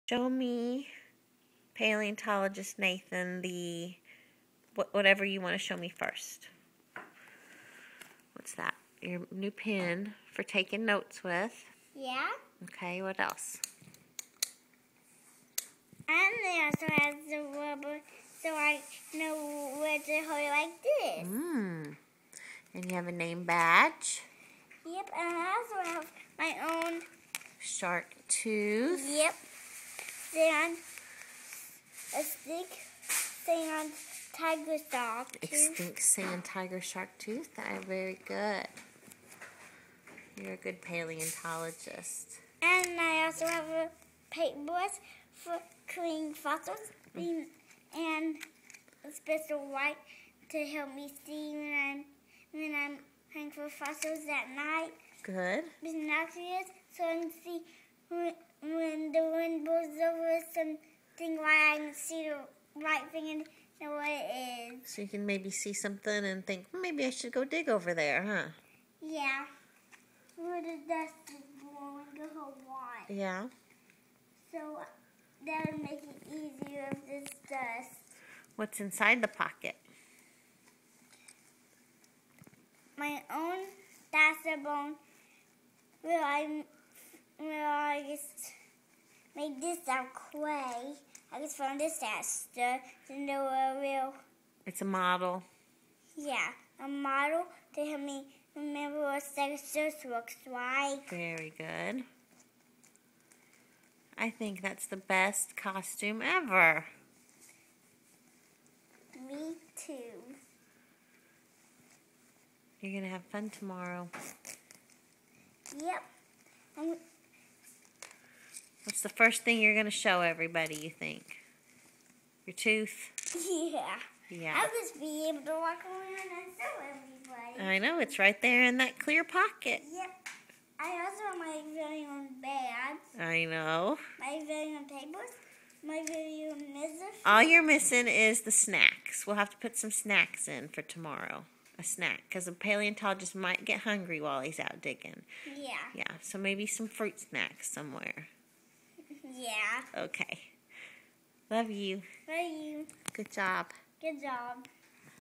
Paleontologist Nathan
Nate's going to be a paleontologist at school tomorrow. He'd super into it. So cute.  -3